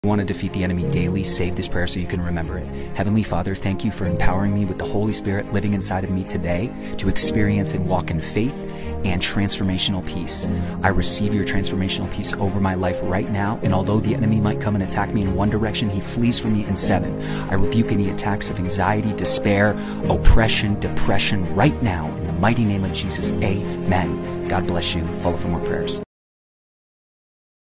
Powerful Prayer for empowering you with the holy spirit!! AGREE WITH ME IN PRAYER
Heavenly Father thank you for empowering me with the holy spirit Living inside of me today to experience walk in faith and transformational peace.
I receive your transformational peace over my Life right now. although the enemy might and attack me in one direction he will flee from me in seven.
I rebuke any attacks of anxiety,despair,oppression,depression right now in the mighty name of Jesus Amen
God Gives You what you can manage